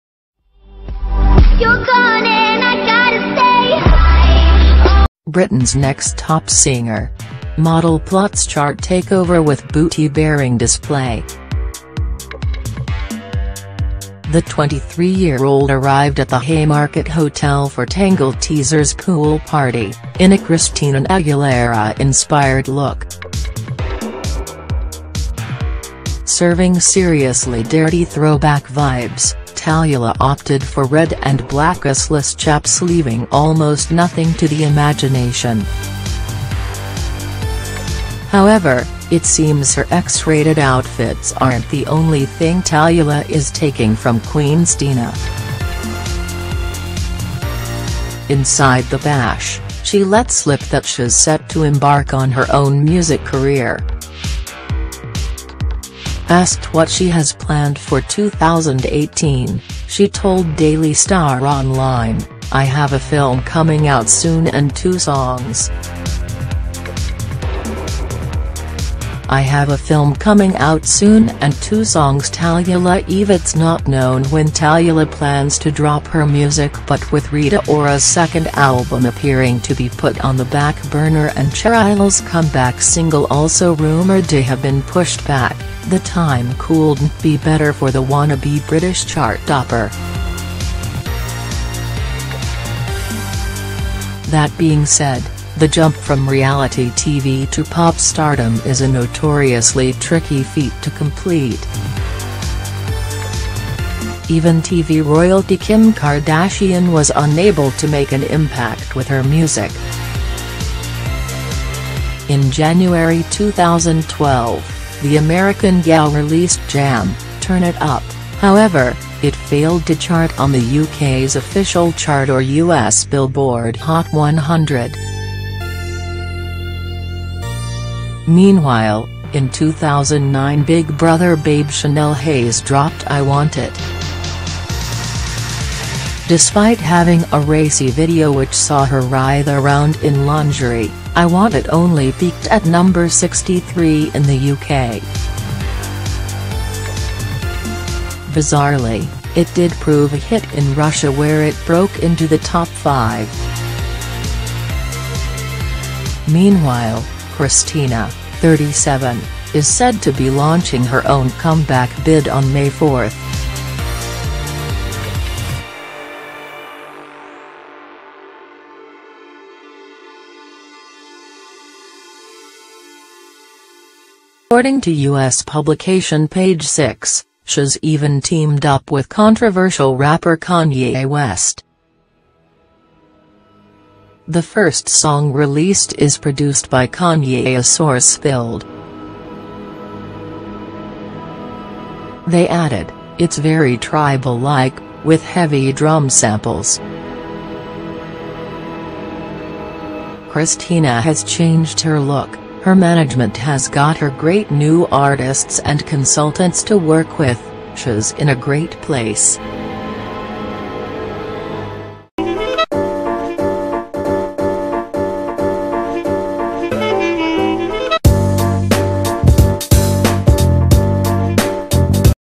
Britain's Next Top singer? Model plots chart takeover with booty-baring display
BRITAIN'S Next Top Model star Talulah-Eve is keen carve out a name for herself in the music industry.